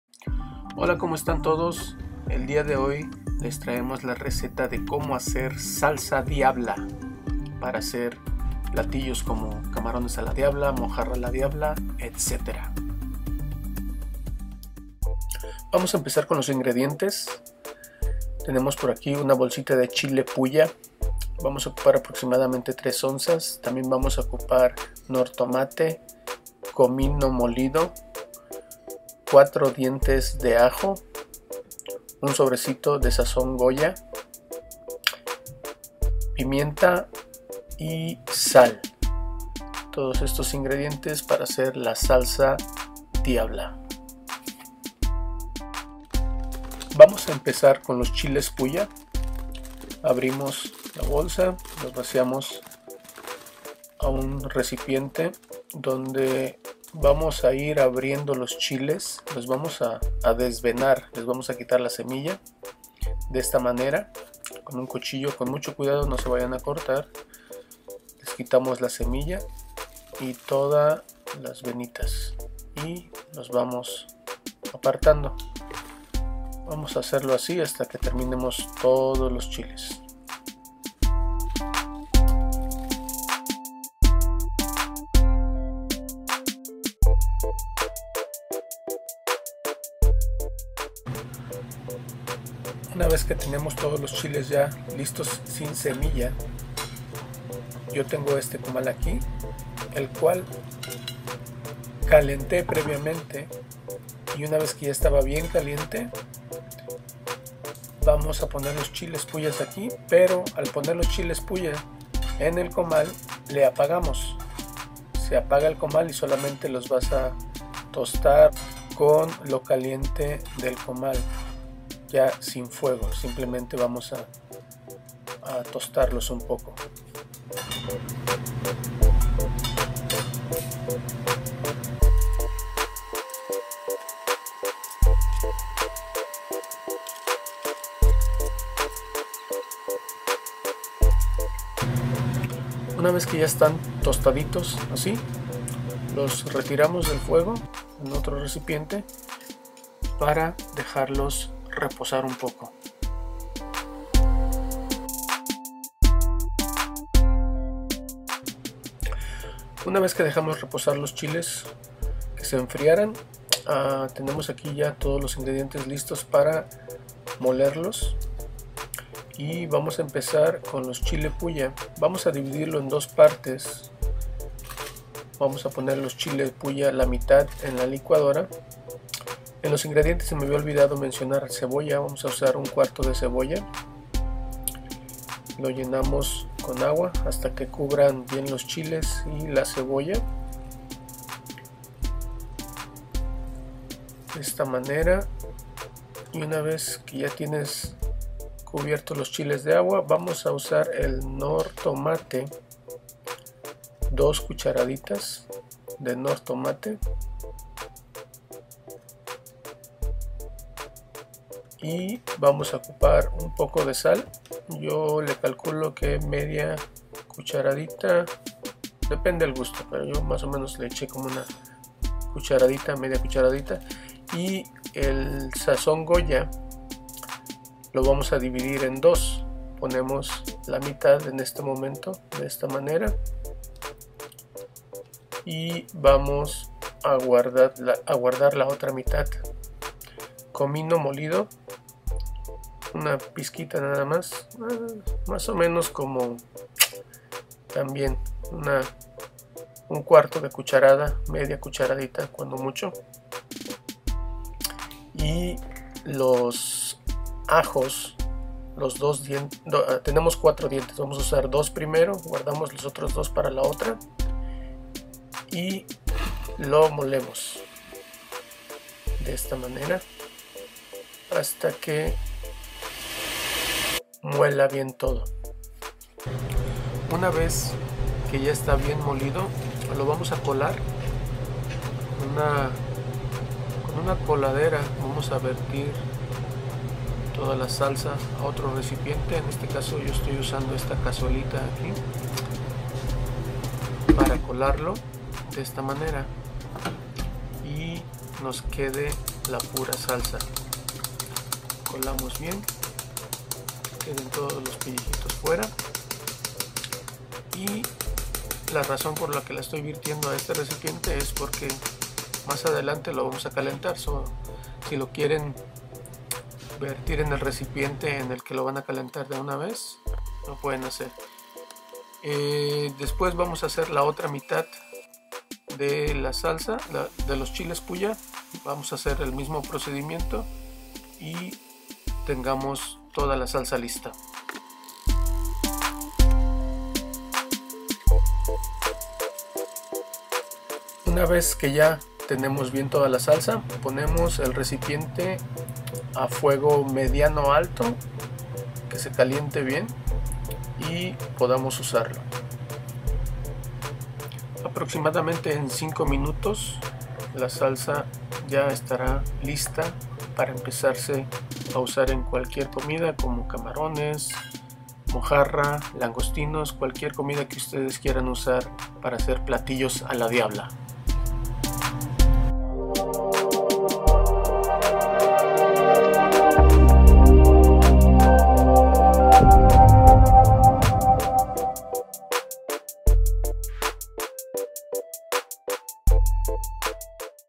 Receta de Salsa a la Diabla
En este video se muestra como hacer Salsa a la Diabla con Chile Puya